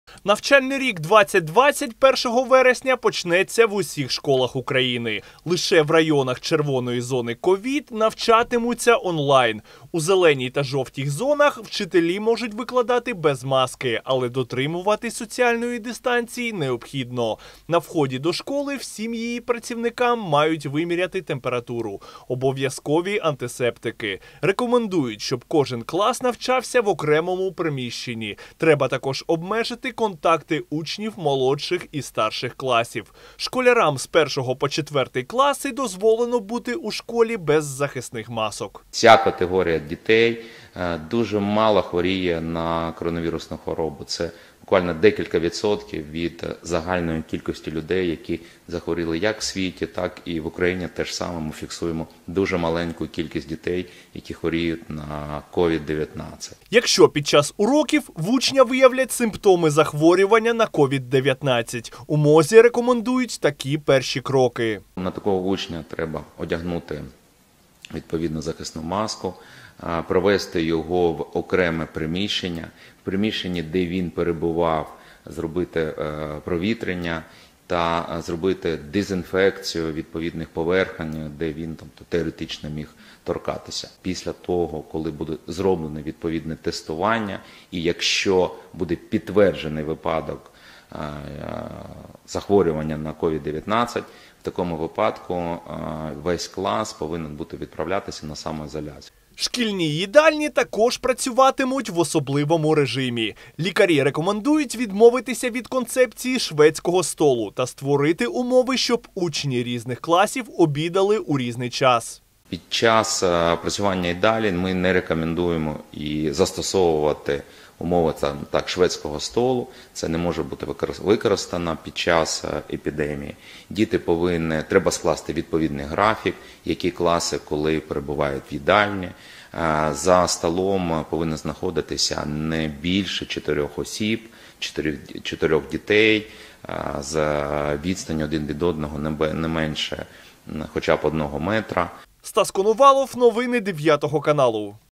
До навчального року під час епідемії COVID-19 у МОЗ розробили низку рекомендацій
До навчального року під час епідемії COVID-19 у Міністерстві Охорони Здоров'я розробили низку рекомендацій. Нові правила для навчання в школах під час пандемії впровадили після консультацій з міжнародними експертами та представниками Міносвіти. Основні пункти особливих умов навчання оприлюднив міністр охорони здоров’я Максим Степанов.